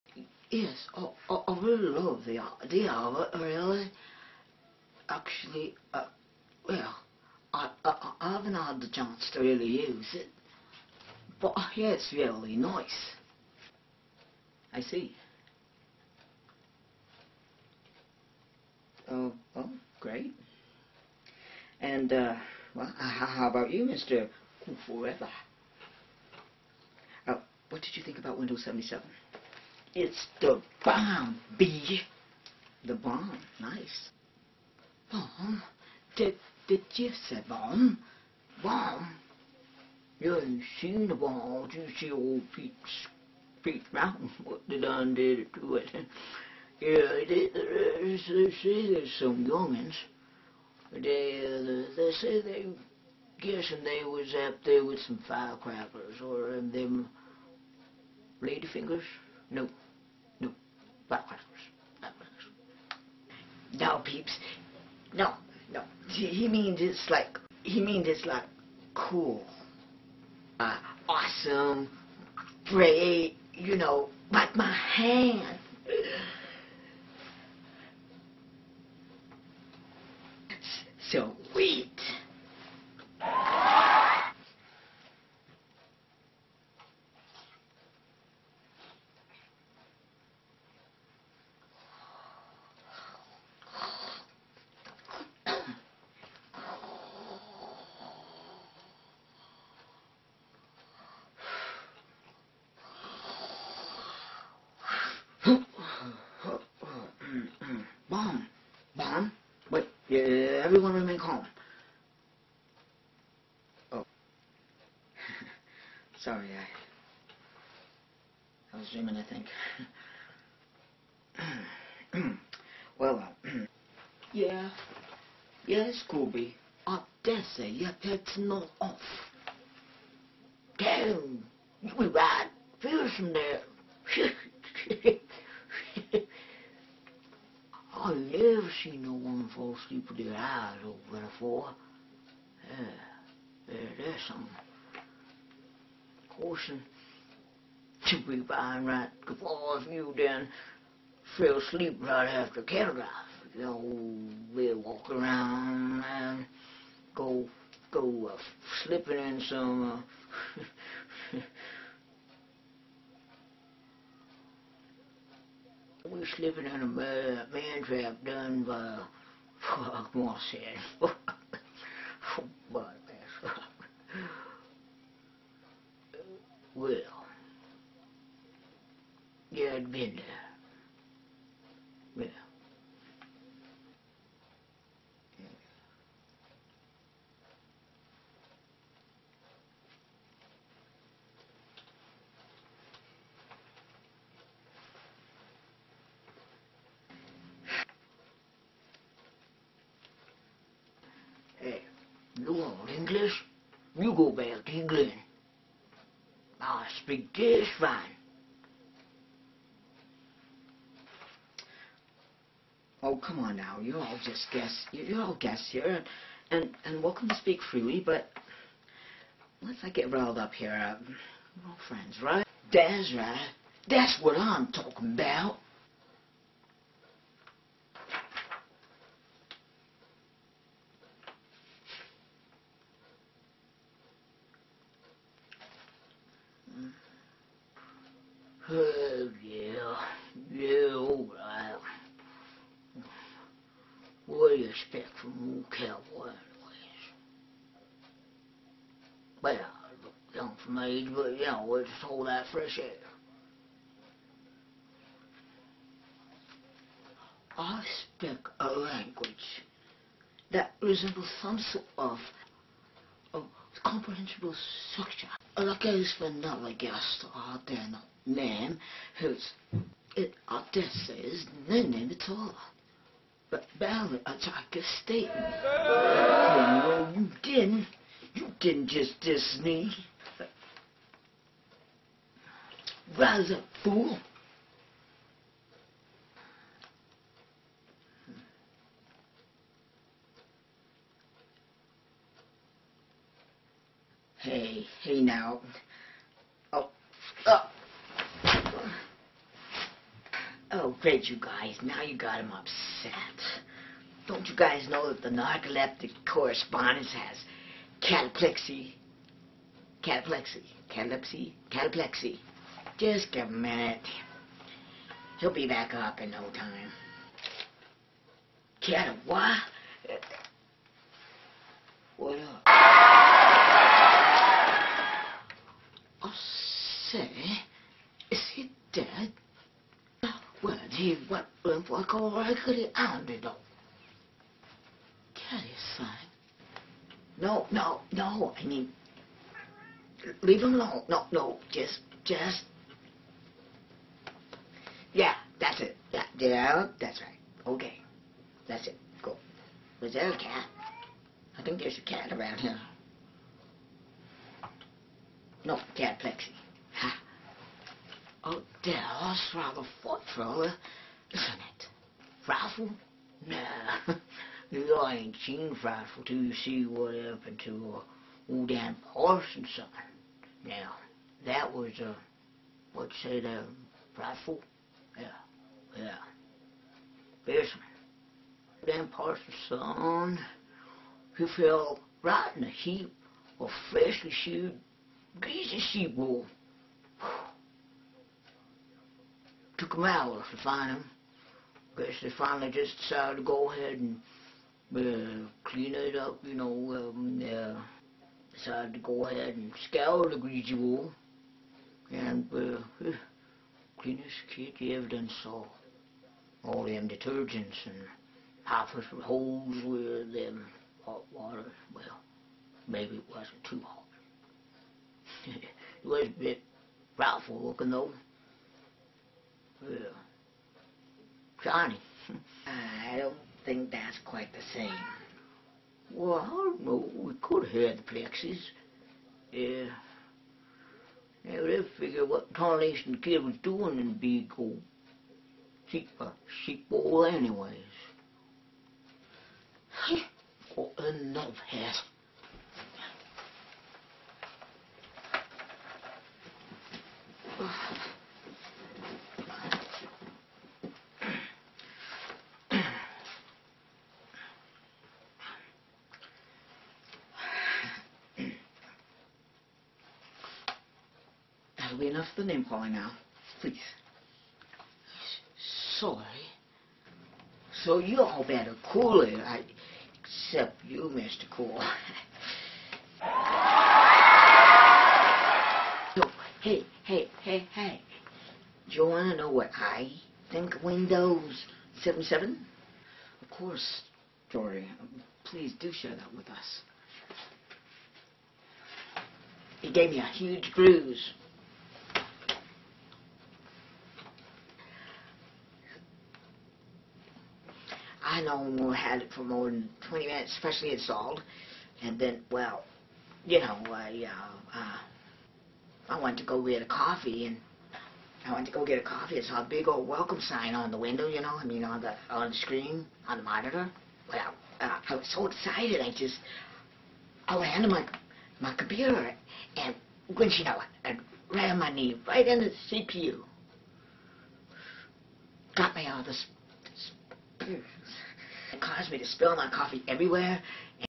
Not another computer operating system! Part 2
Stereotypical Topics Episode One Part 2 of 3
Today's Topic -  Wendows 77 (Not another computer operating system!)

A comedy featuring stereotypical topics discussed by stereotypical guests that are not so stereotypical.

Tribal Rose & TecZ Music Productions Presents ...
Stereotypical Topics Episode One
With your Host...
The Narcoleptic Correspondent (No disrespect to anyone else with Narcolepsy, gotta laugh at myself once in a while)
Played by, Dalton C. Teczon (yes, I also have cataplexy)

Any resemblence to real life persons is merely coincidental.

All characters played by, Dalton C. Teczon;

Mr. Snowburn, the Cattle King
Mr. Cool 4 Ev'r
Belstone, our British Sudokar Champion
Drory, The Studio's Computer Technician

Script, Editing, Cinematography, Makeup, Direction and total creation by, Dalton C. Teczon

All Rights Exclusive - Copyright March/April 2010 - Dalton C. Teczon